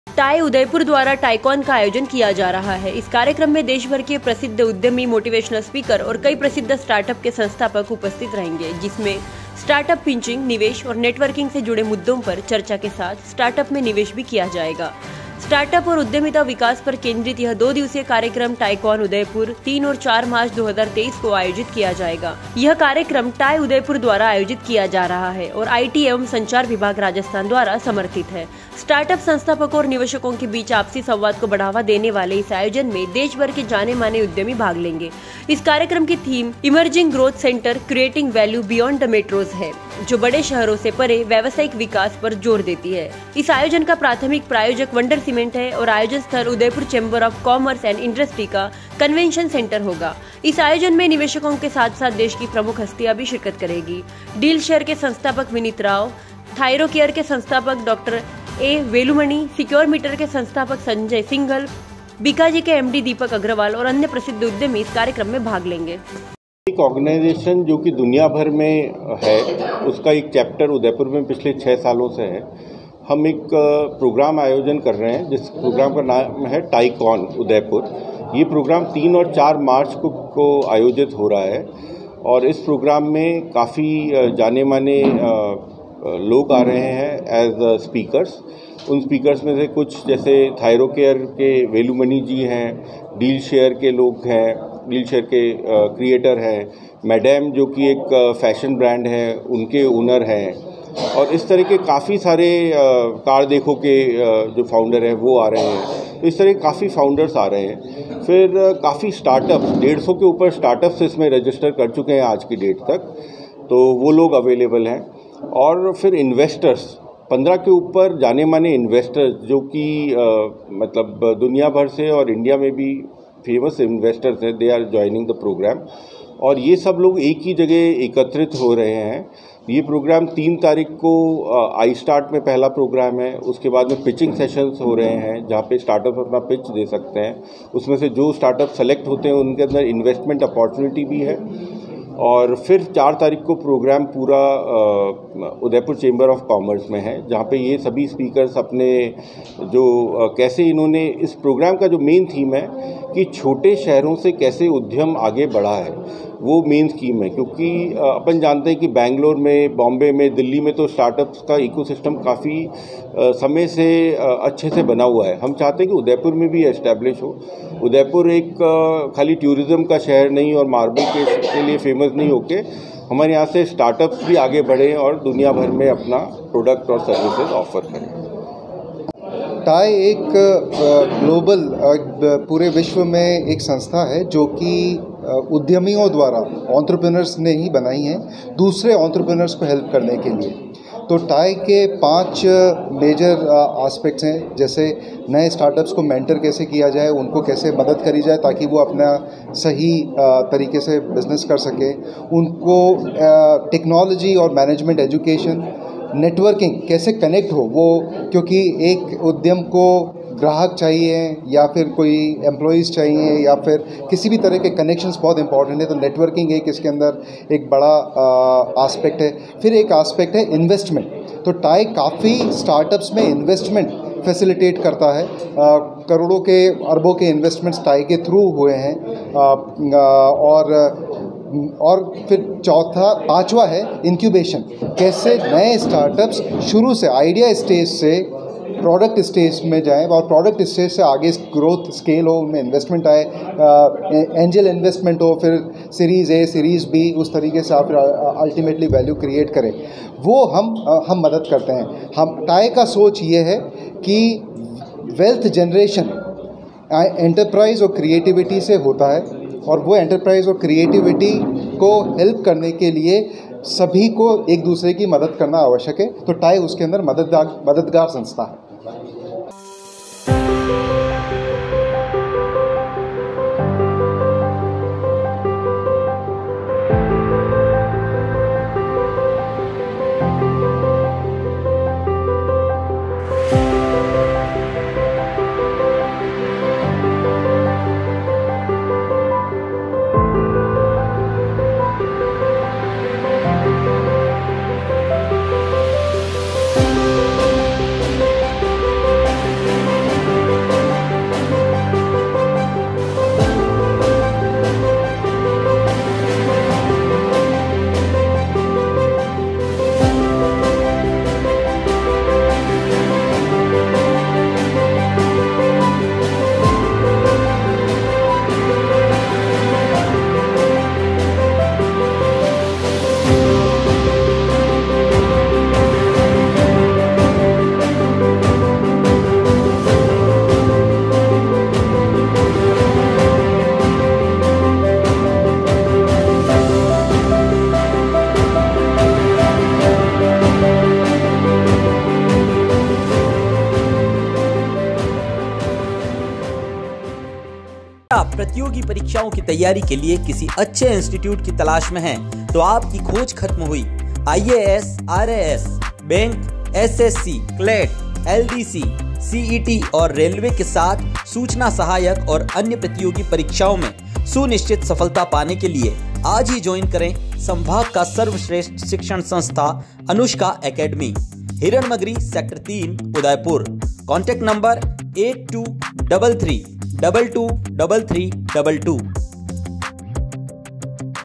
देशभर के प्रसिद्ध उद्यमी, मोटिवेशनल स्पीकर और स्टार्टअप संस्थापक लेंगे भाग
3 और  4 मार्च को देशभर के प्रसिद्ध उद्यमी, मोटिवेशनल स्पीकर और स्टार्टअप संस्थापक लेंगे भाग